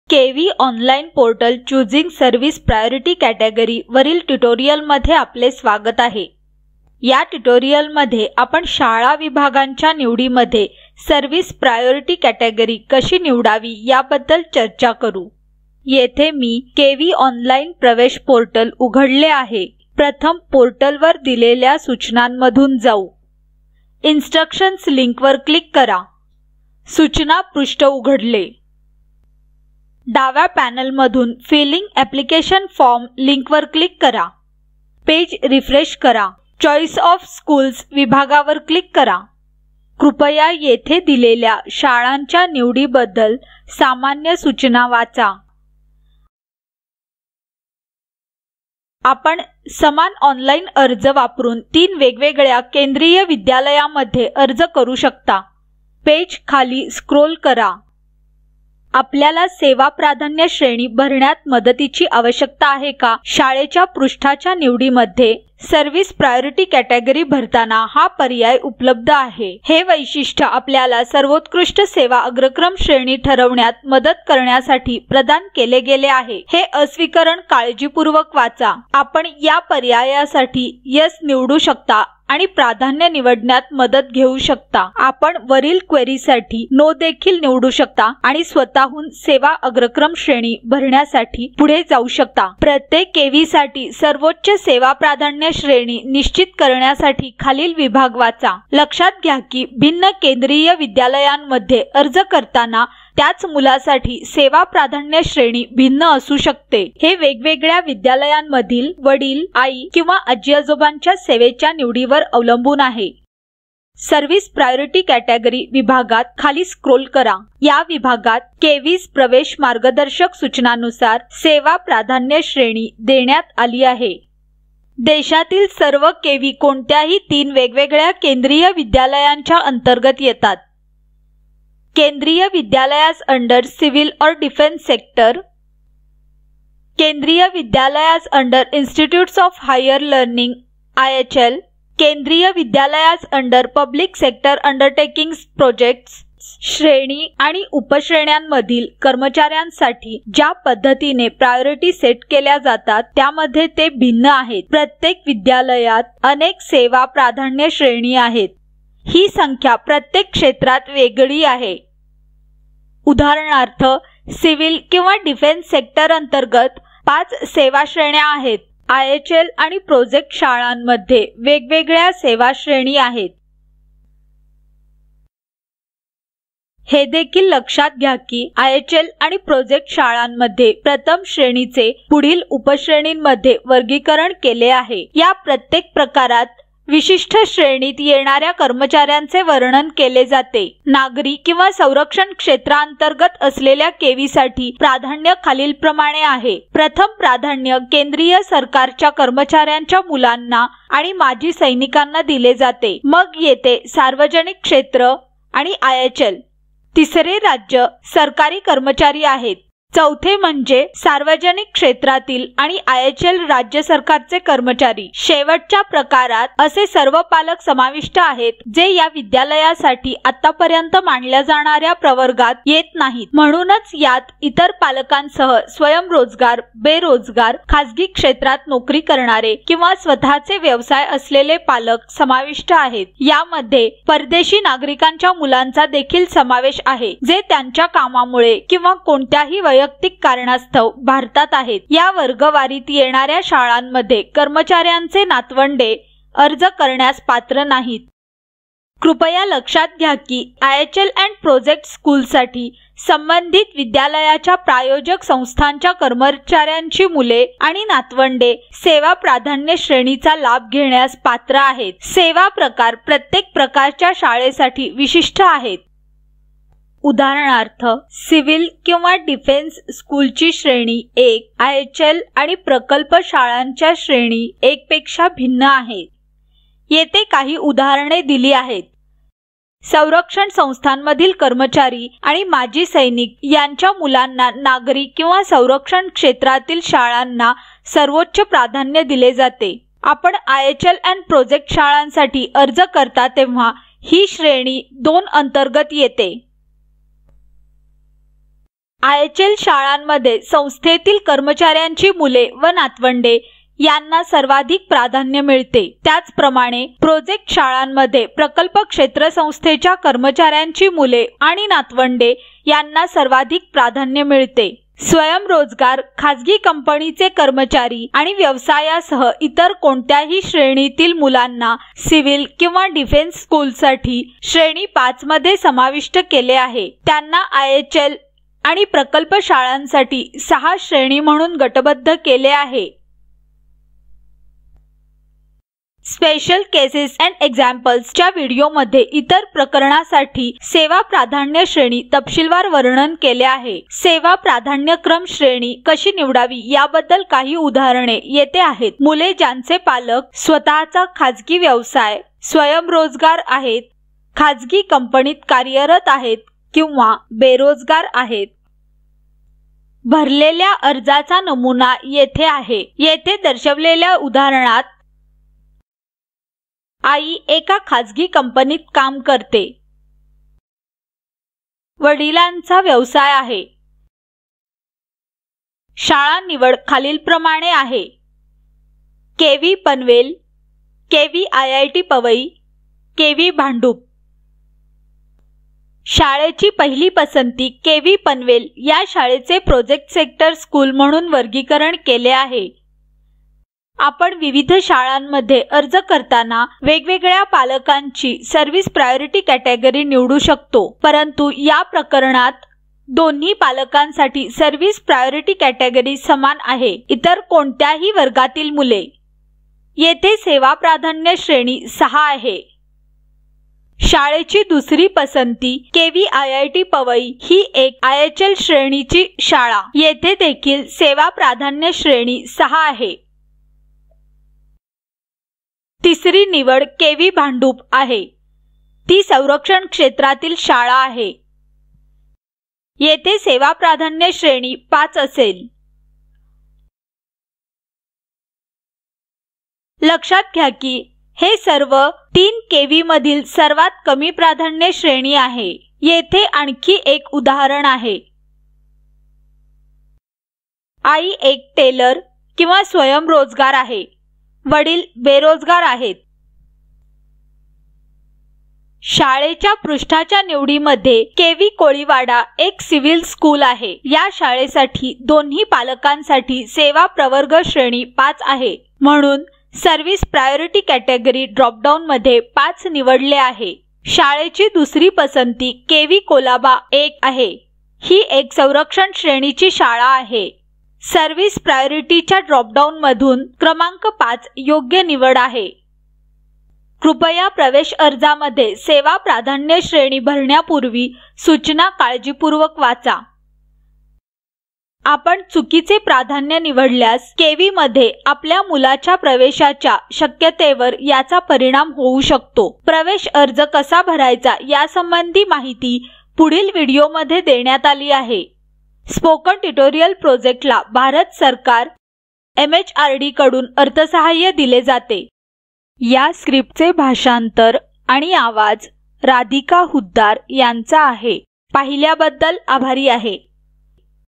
KV Online Admission Portal: Choosing Service Priority Category (Marathi)
This video gives information on how to choose Service Priority Category in filling up the Choice of Schools section.
शैक्षणिक वर्ष  2021-22 मध्ये केंद्रीय विद्यालयांमध्ये इयत्ता पहिलीच्या प्रवेशासाठी,
कृपया 30 मार्च 2021 रोजी किंवा नंतरच्या तारखेतील चर्चा आणि टिप्पण्या चर्चा मंचमध्ये तपासा.